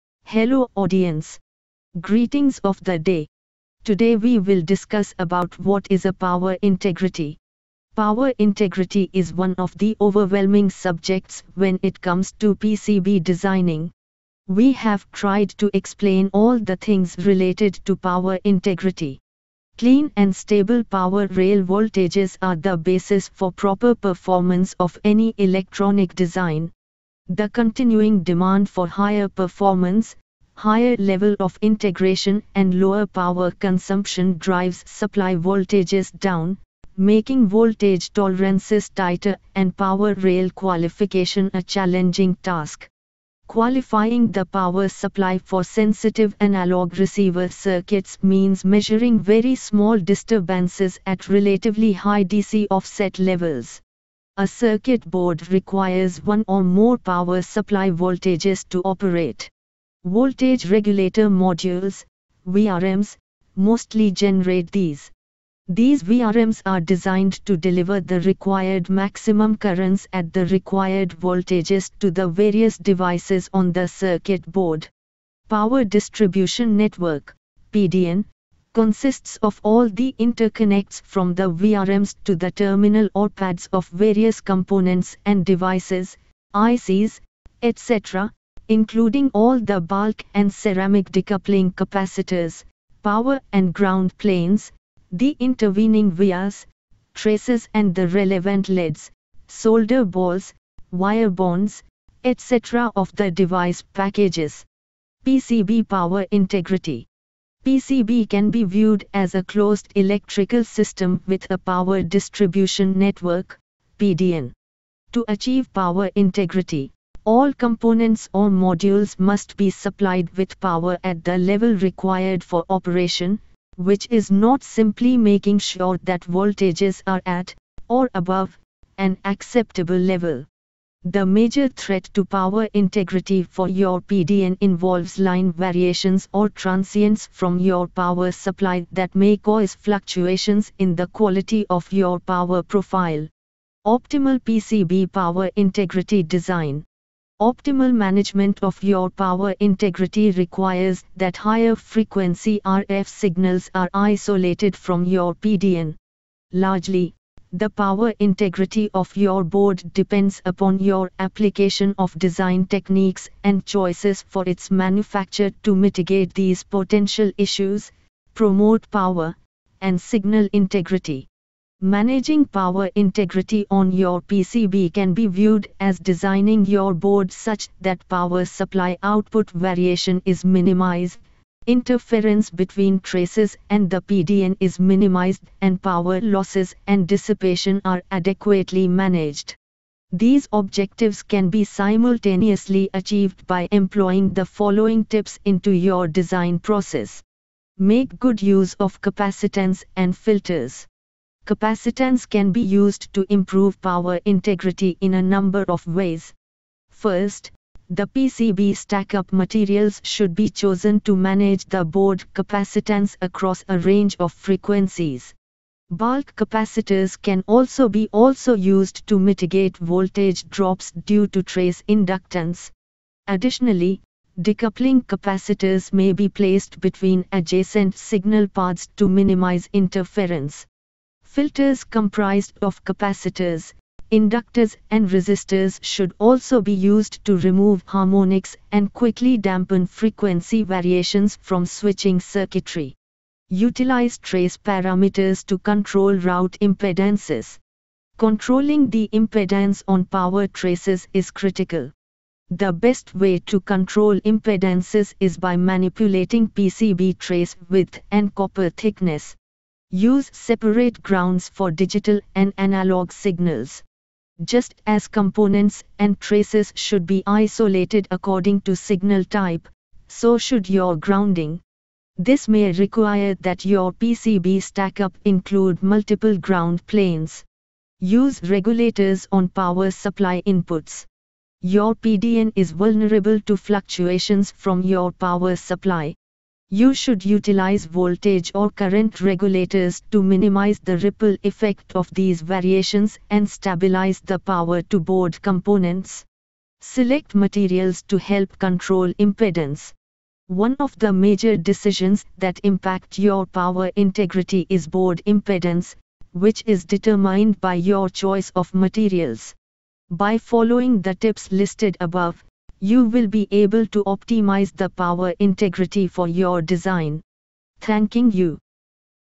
What is a Power Integrity?
Power integrity is one of the overwhelming subjects when it comes to PCB designing.
We have tried to explain all the things related to power integrity.
Clean and stable power rail voltages are the basis for proper performance of any electronic design.
The continuing demand for higher performance, higher level of integration and lower power consumption drives supply voltages down, making voltage tolerances tighter and power rail qualification a challenging task.